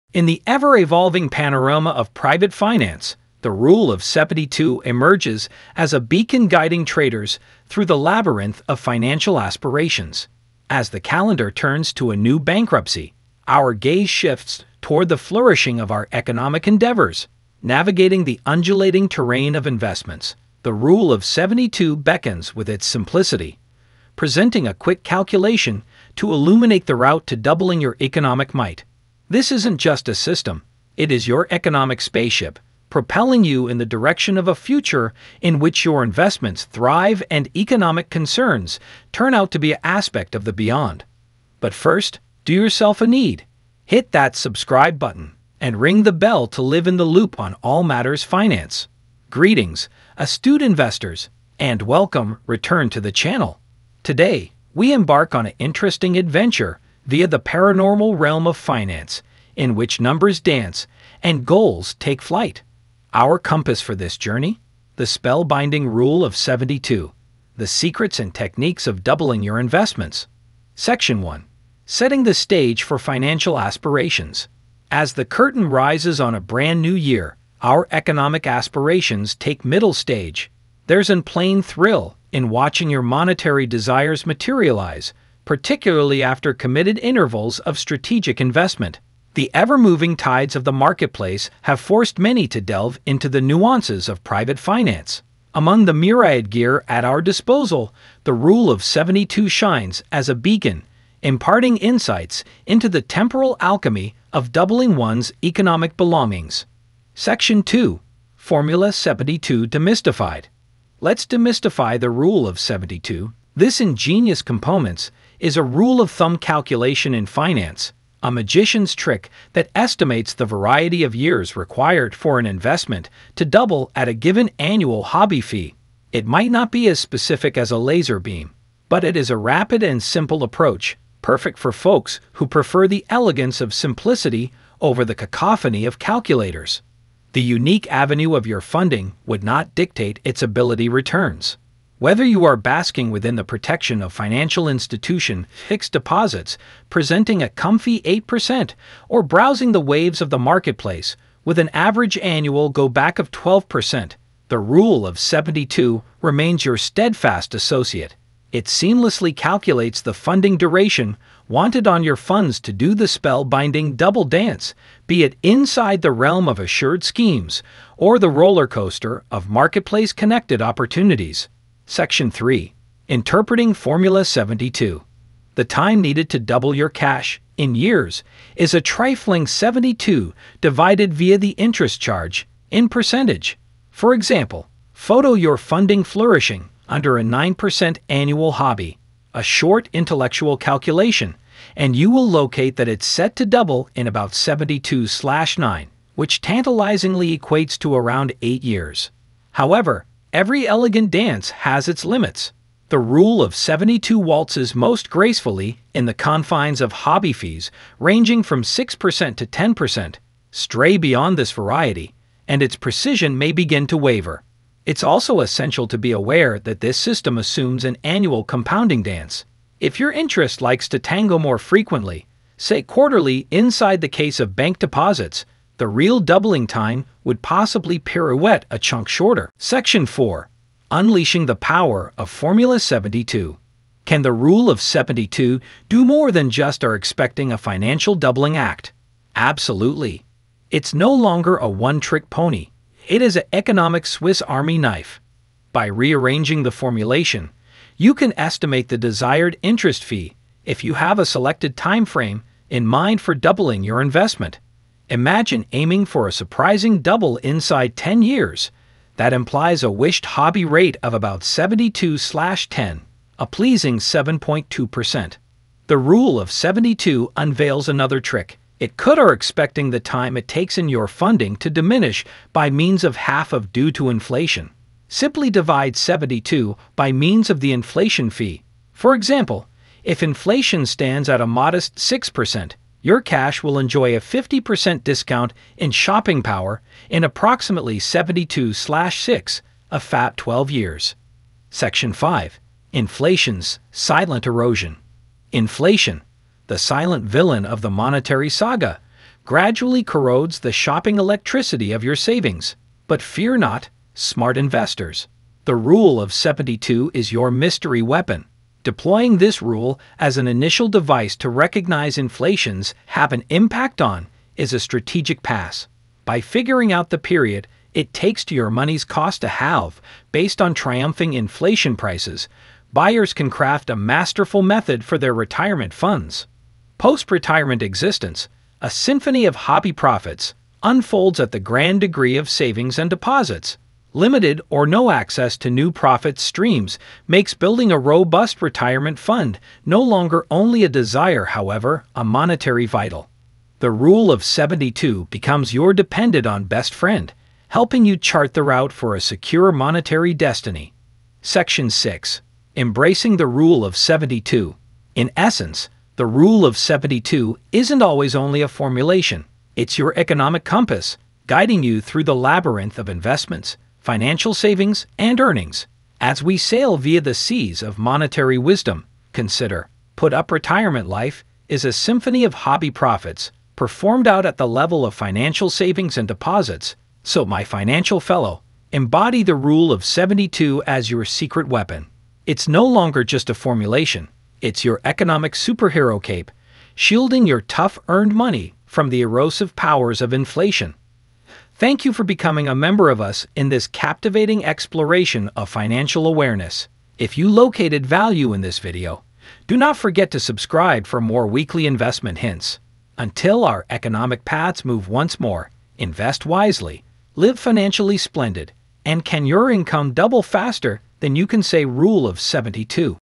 Rule Of 72 - This Formula Will Change Your Life
In this eye-opening video, we delve into the powerful concept of the Rule of 72. Discover how this simple formula can revolutionize your financial perspective and help you achieve your goals faster than ever before.
Whether you're saving for retirement, investing in stocks, or planning for the future, understanding the Rule of 72 is a game-changer. Join us as we break down the math behind it and explore real-life examples that will leave you inspired and motivated.
Don't miss out on this life-changing formula – watch now and unlock the secrets to financial success!